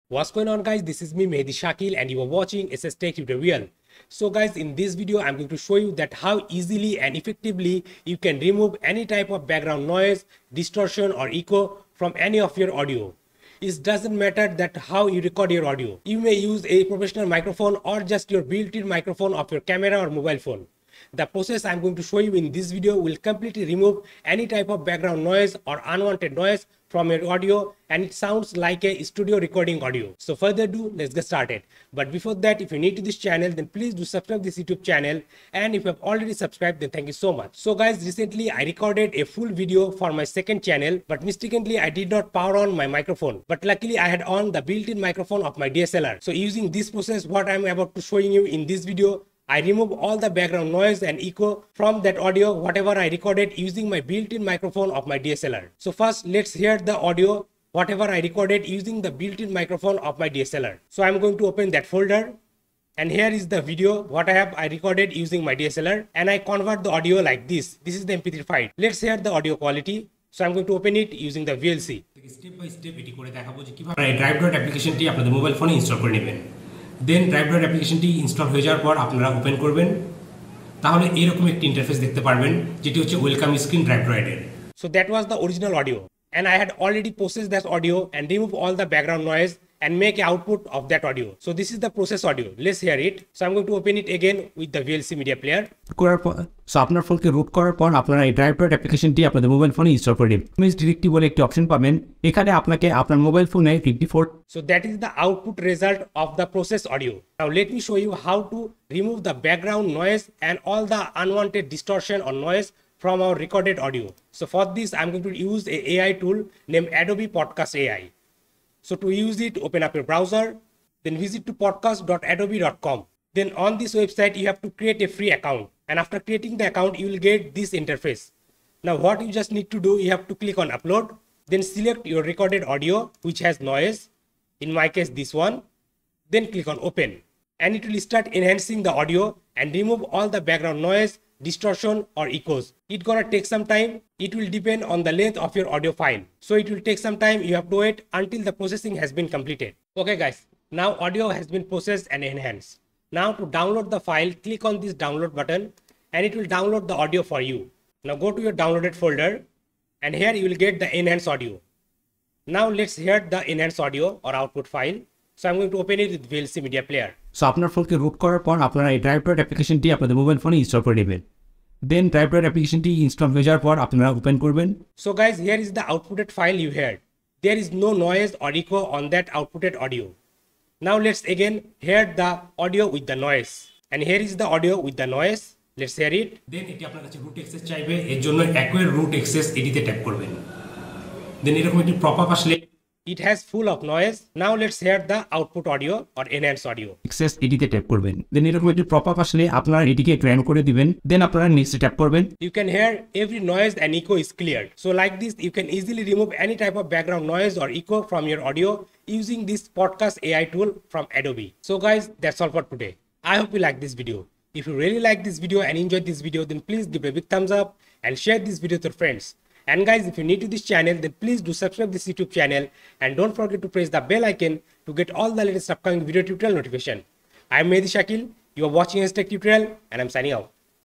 How To Use Ai To Turn Trash Audio Into a Professional Audio
Turn Trash Audio into a Professional Studio Recorded Sound! How To Remove Any Noise From Any Audio and Make It Sound Professional? No matter how you record your audio, what microphone you use, \you can make any trash audio sound like studio recorded professional audio. You can remove any background noise , echo and distortion from your audio with just a simple click. In this video, I'm going to show you the simplest way to remove Any Noise or background noise From Any Audio and Make It Sound Professional! I hope you will like this video.